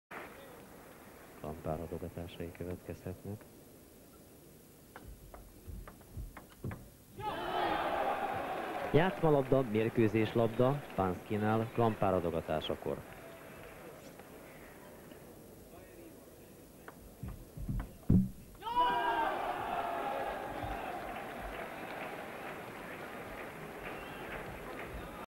Klampár Tibor vs. Jindřich Panský - European Table Tennis Championship 1982 final
Hungary - Czechoslovakia 5:3 European  Table Tennis Championship 1982  Budapest